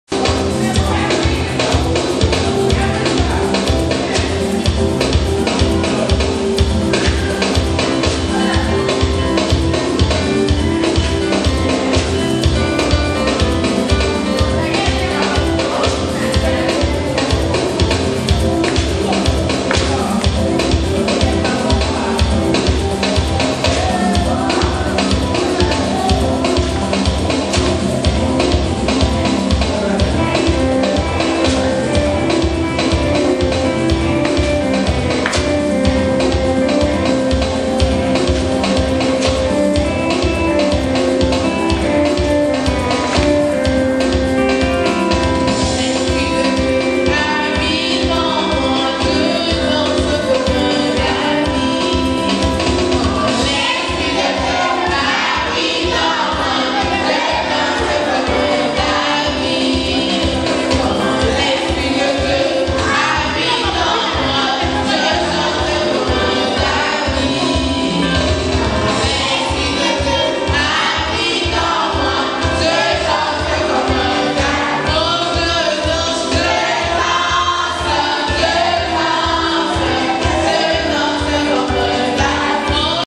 Africa Imports | Visit to Congo - Video of children dancing part 1
Wayne and Randy Kiltz visited the many orphanages Africa Imports supports on the weekend of October 6, 2012. Here is a video of some children we support dancing during service.